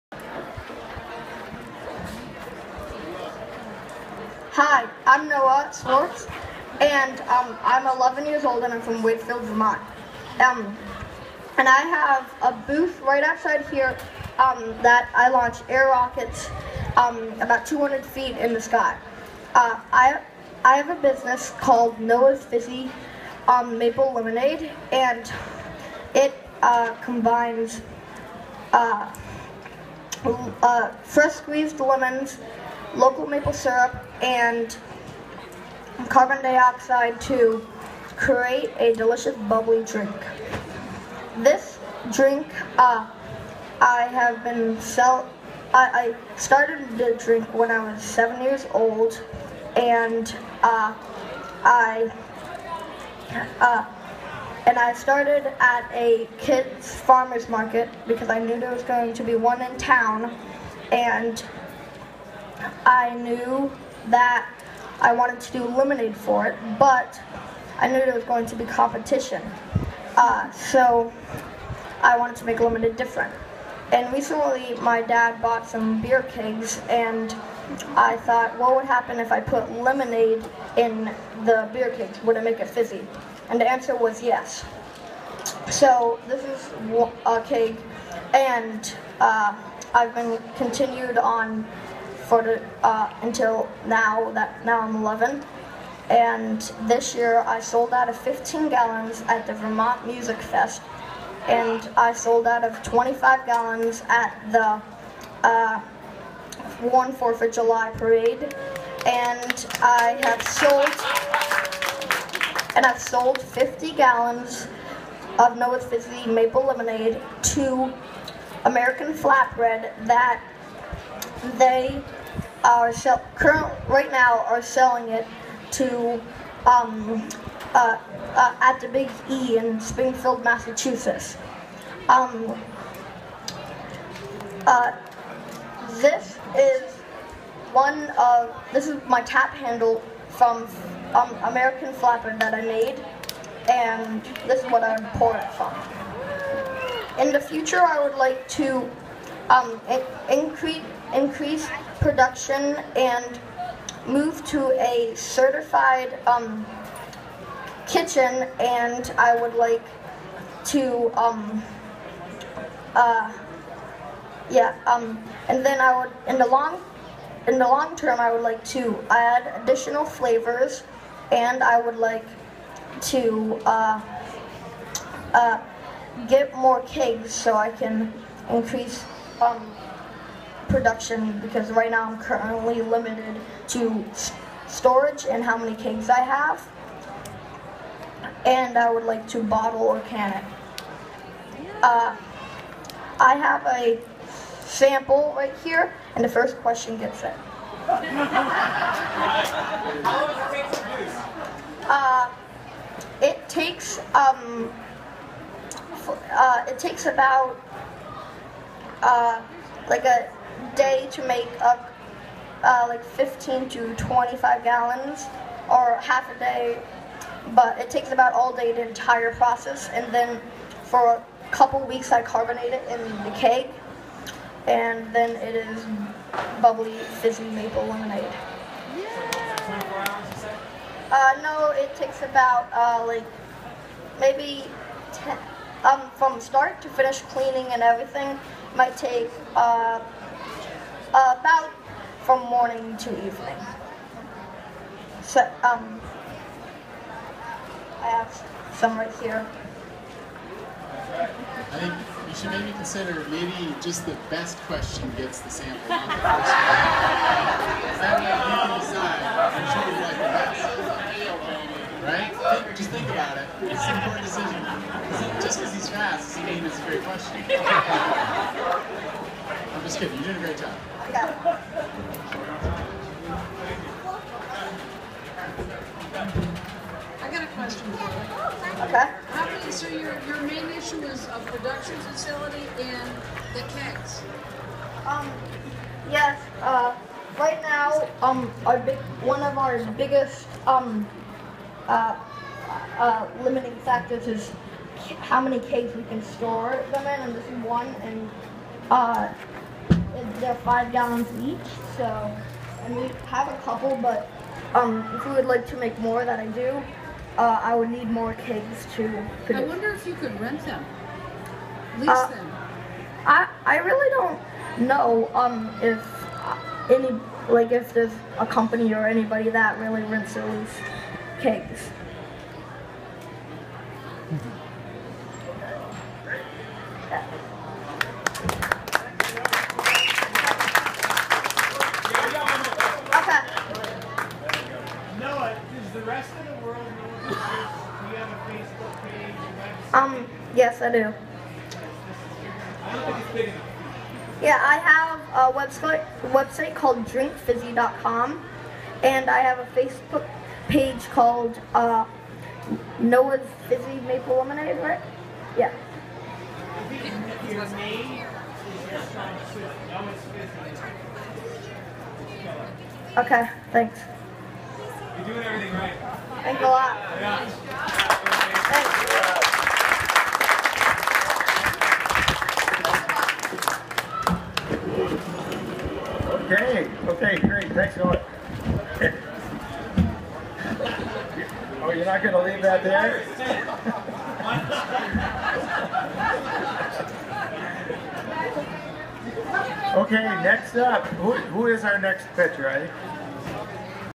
Noah's Fizzy presentation at the FreshTracks Capital Road Pitch
Here's Noah's pitch at the FreshTracks Capital Road Pitch competition, located at the 2015 Champlain Mini Maker Faire. The "business biker" group of experienced entrepreneurs, investors and advisers listened to 6 pitches, no longer than 3 minutes each, describing an invention, the problem it solves, and how one intends to introduce it into the market. Noah received some very valuable feedback and first place.